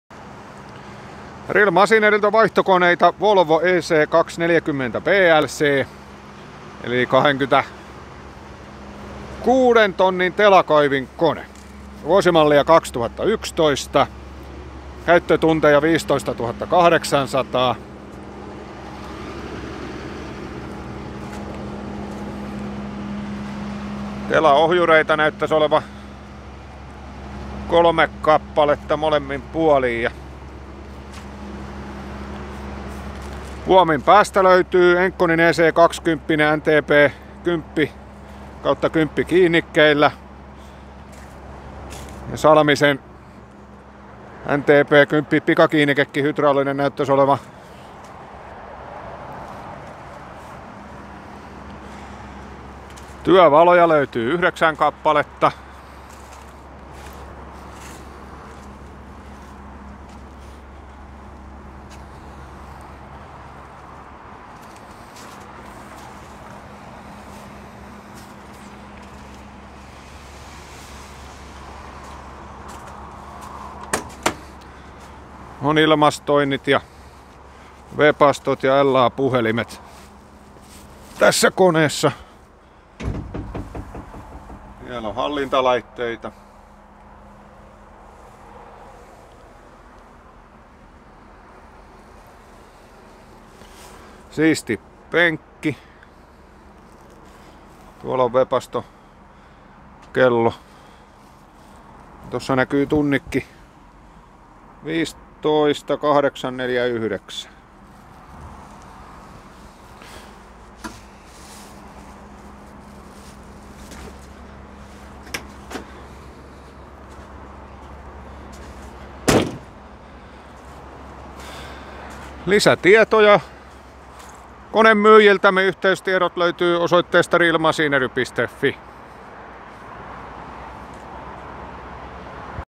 Volvo EC240BLC, vm.2011, 15780h -80431
- Engcon EC20 tiltti NTP10/NTP10 + propo
- Puominpään hydraulinen liitin Terästyö T. Salminen Oy
- Rasvari
- Webasto
- Pikakiinnityshydrauliikka, vasarahydrauliikat
- Työvalot 9 kpl
- Majakkatappi
- Radio
- LA-puhelin
- Ilmastointi
- Sammutin
- Pohjapanssari
- Kaivuuvarren pituus 2950 mm
- Telalappujen leveys 700 mm

- Telaohjurit 3 + 3

RMV80431